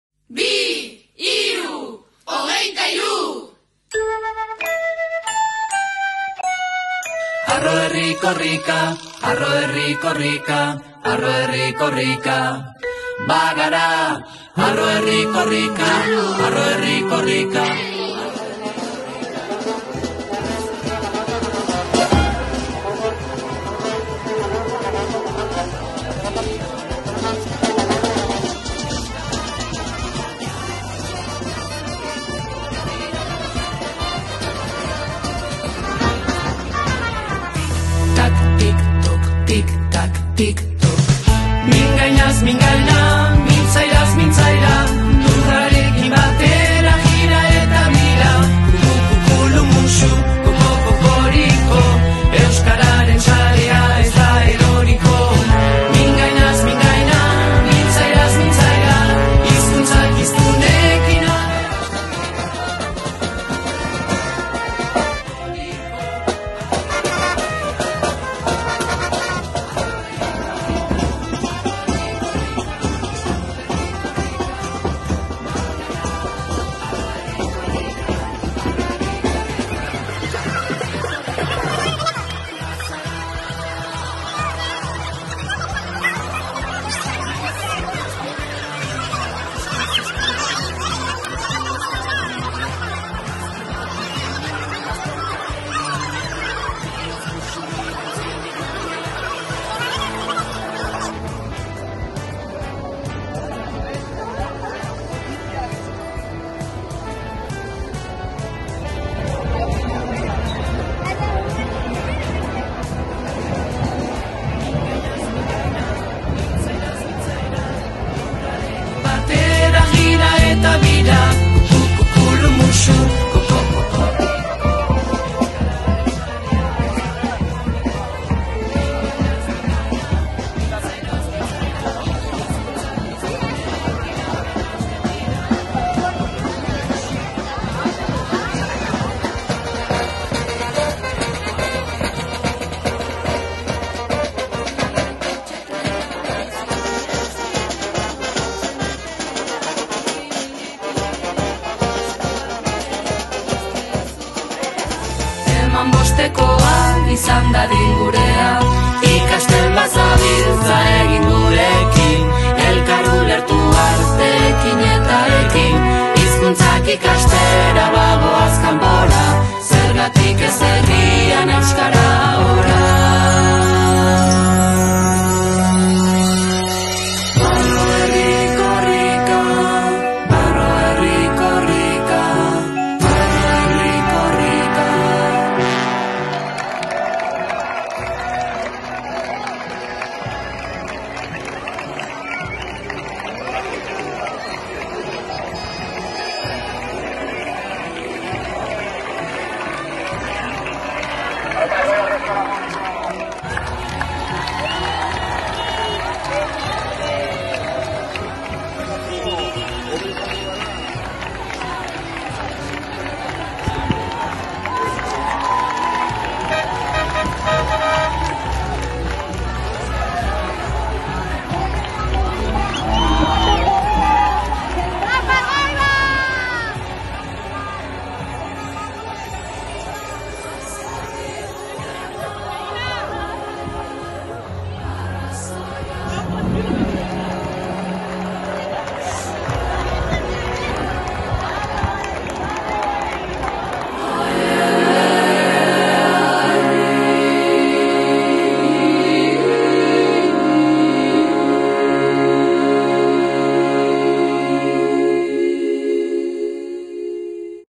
Elgoibar BHI ere HARRO korrika!
23. Korrika hartu du Elgoibarrek gaur, 2024ko martxoaren 20an, eta txaranga eta guzti hasi gara gure ikastetxean ospakizunetan. Olasoko industria-gunean batu gatzaizkio Korrrikari eta hortik aurrera bakoitzak geure - lasterkako gaitasunaren araberako- aletxoa jarri dugu.